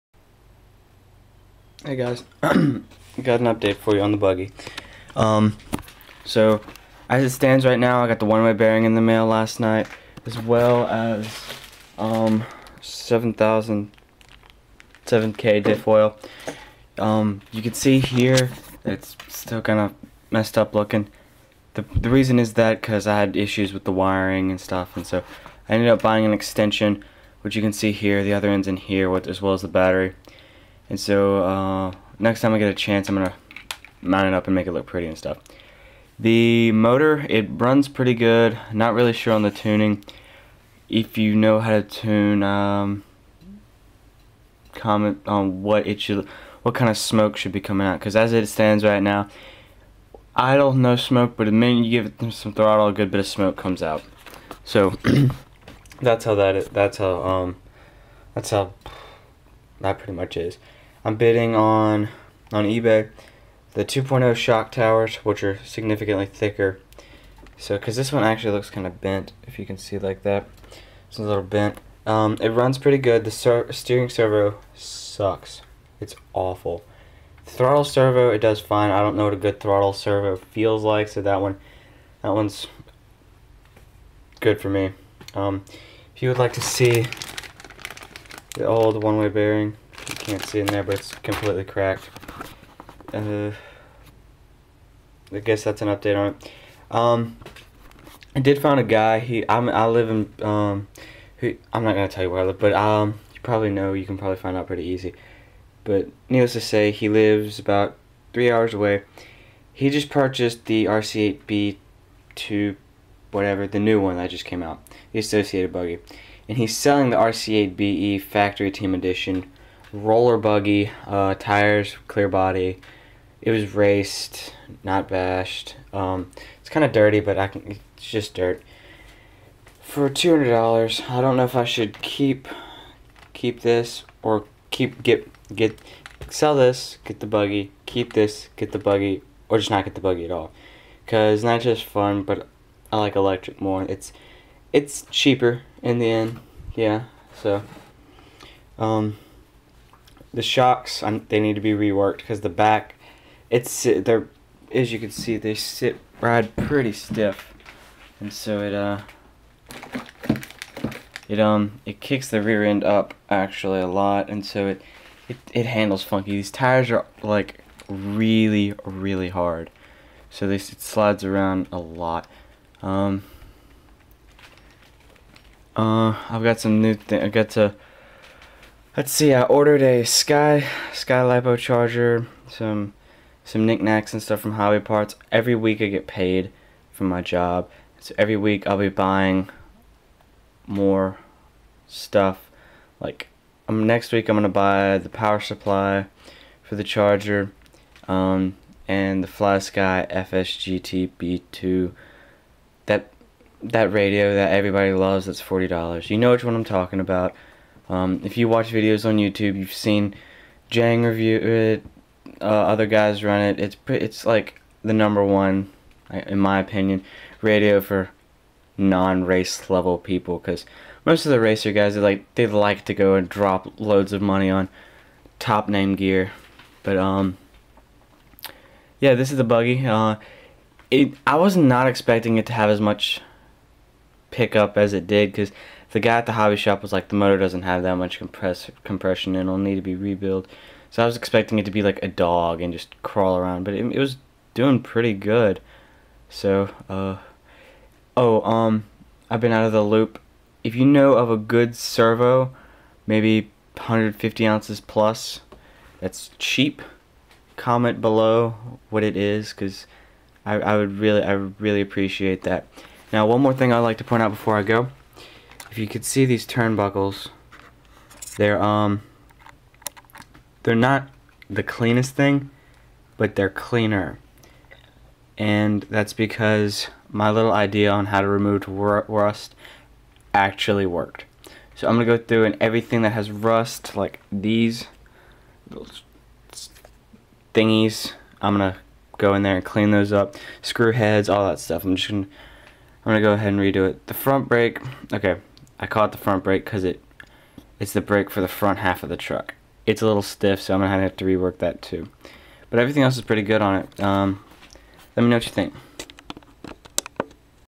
losi update 8ight update part 3
I got the motor running. need advice on cheap servos. should i keep this buggy?

at the Cajun classic i had a chance to talk with a Associated sponsored driver and he was tellnig me about all the advantages the rc8.2be has over the rc8be and so i might just purchase the kit new... let me know what you guys think i should do.